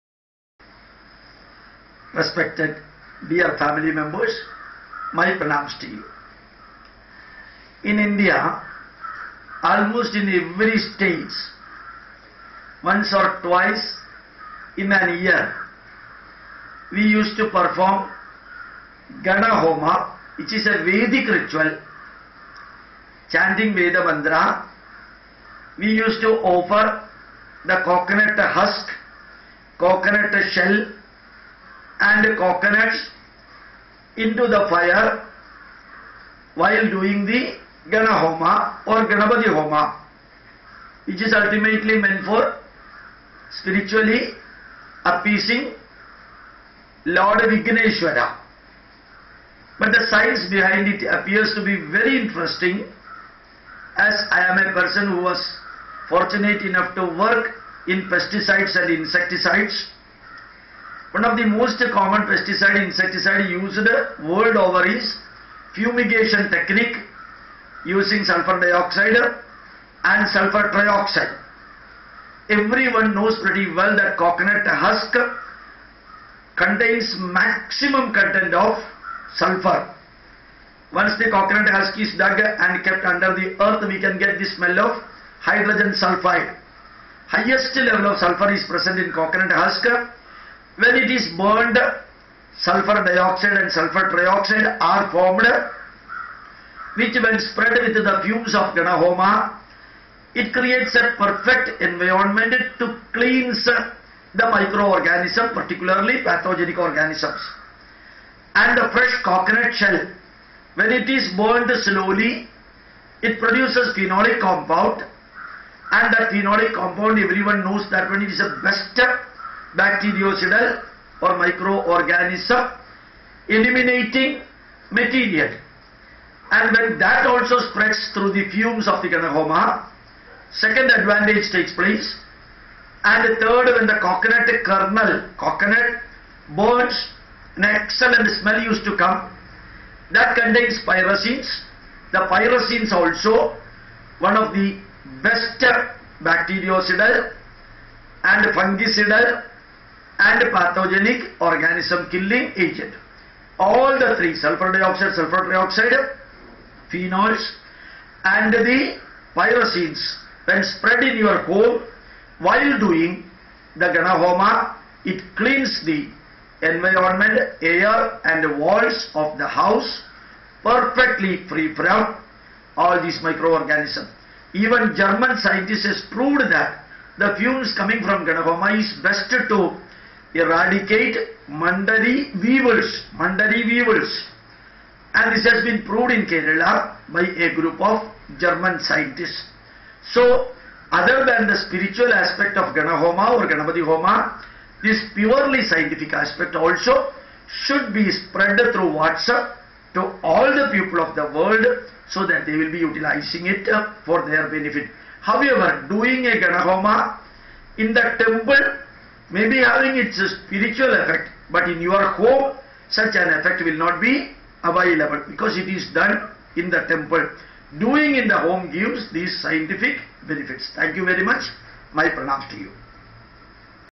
340. SCIENCE BEHIND hindu ritual GANA H0MA -ENGL(08-12-15)
During the havan / homa sulphur dioxide, sulphur trioxie, phenols and pyrazines are produced which spread in the home through the smoke of the havan which acts as a good  microcidal fumigation system